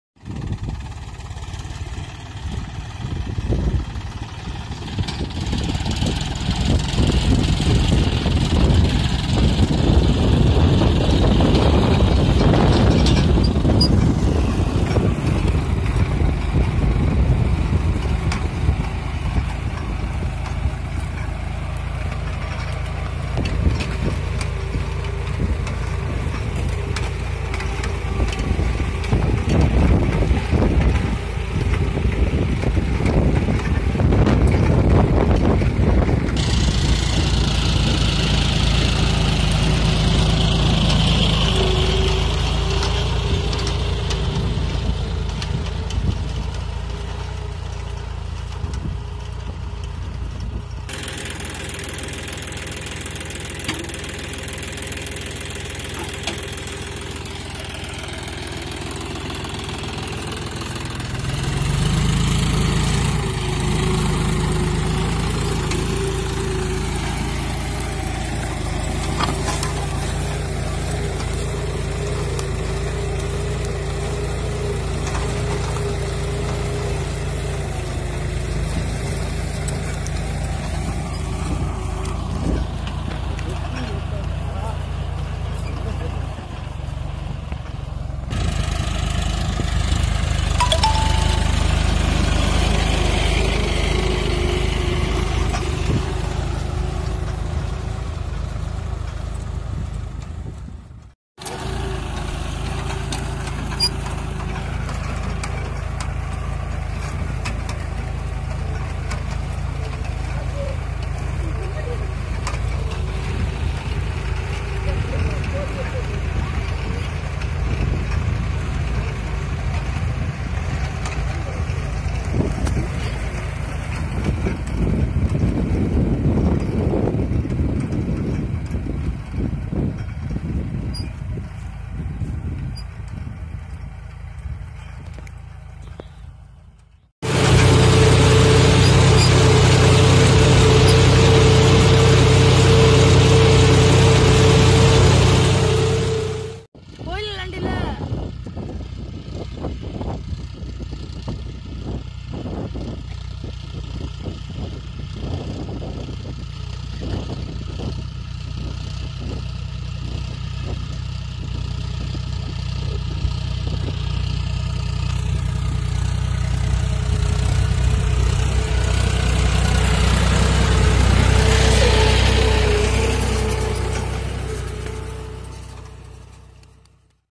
John deere 5105 4wd disc plough| performance new model 5105
In this video performance of new disk plough is viewed and johndeere5105 4wd is best for all the wok of single former and also big former to cultivate land.

It give some knowledge to youngster's into Way of seeding through manual method using tractors and hard work of former.
MORE DETAILS ABOUT TRACTOR
Shop on online Amazon (support us:)
Daily Deal of the Day are listed ( best offered products) in Groups
To Join: